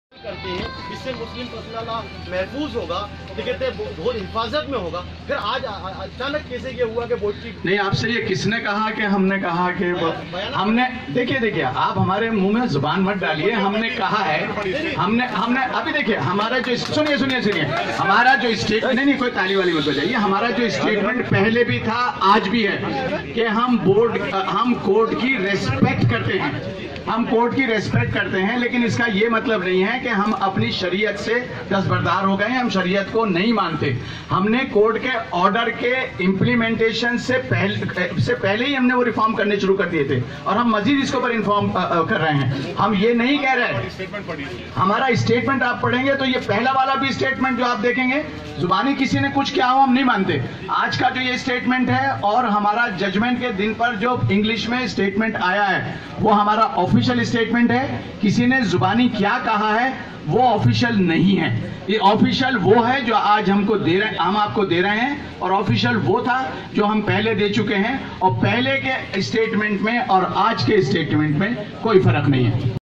Asad uddin owaisi ne reporter Shahid kamil ke sawal ko tala, baji taliyan to roka
Reporter Shahid KAMIL ke sawal per AMPLB ke kamal farooqui ne kaha hamre muh me zaban mat daliye